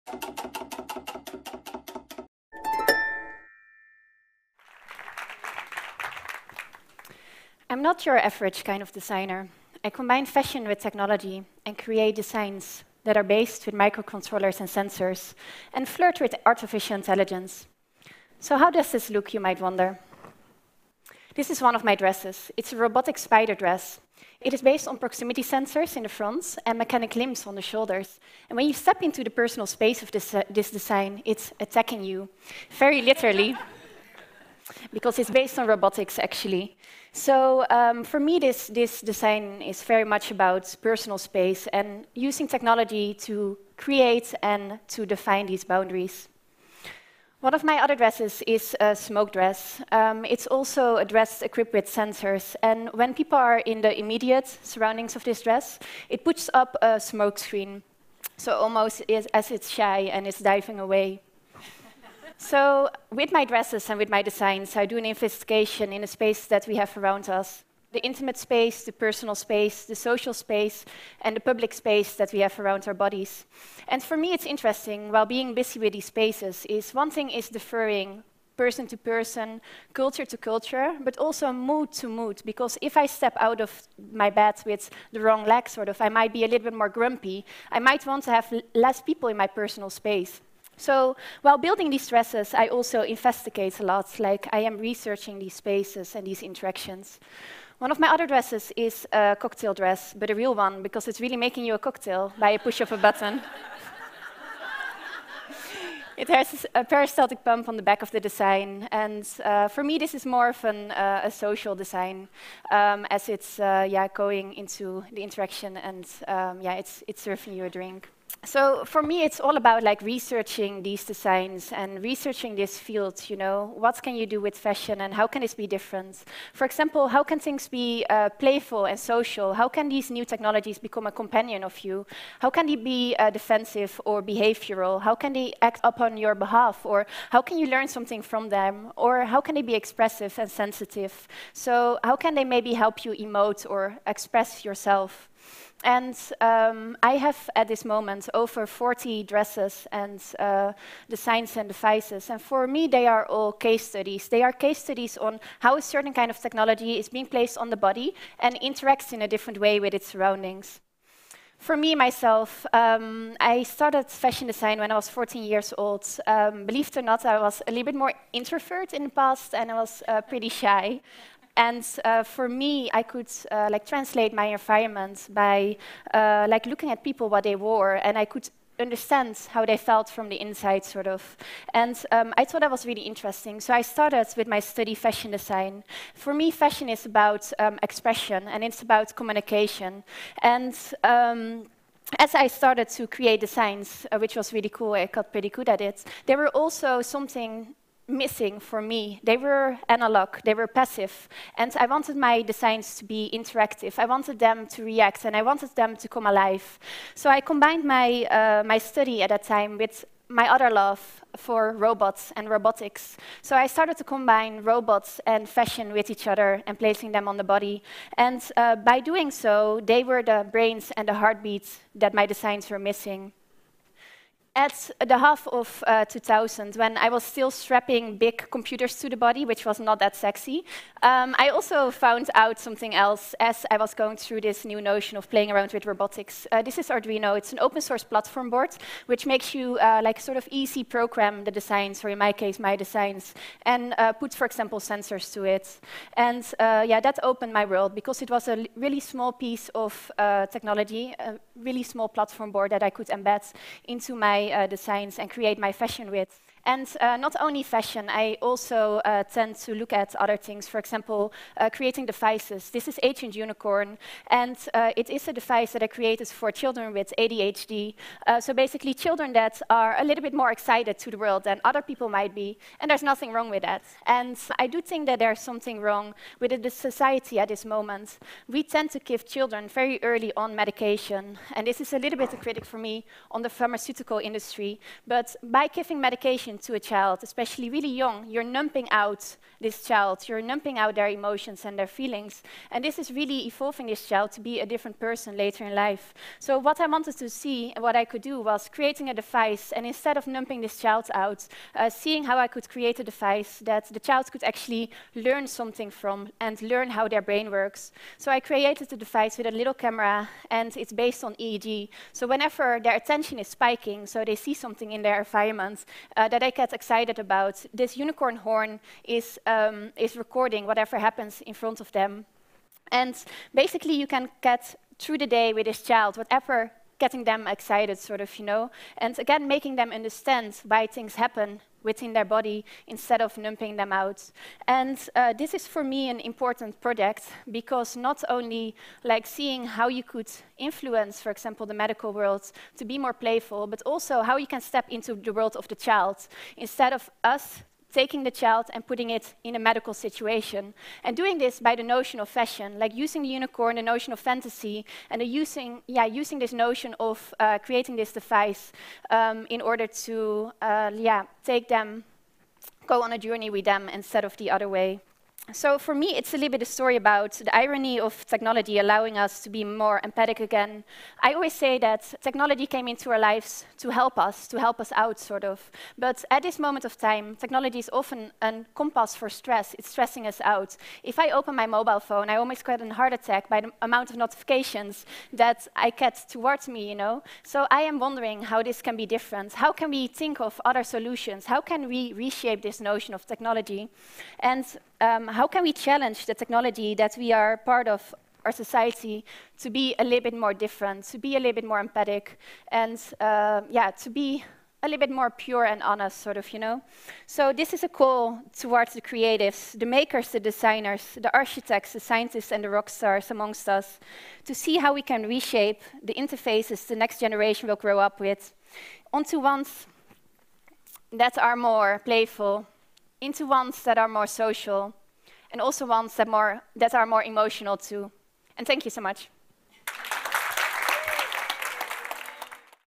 Anouk Wipprecht: Clothing as emotional interface
Anouk Wipprecht is a Dutch designer and engineer whose work combines fashion, robotics and technology in what she calls “technological couture.” In this talk, she walks us through some of her designs and how they help broadcast our feelings.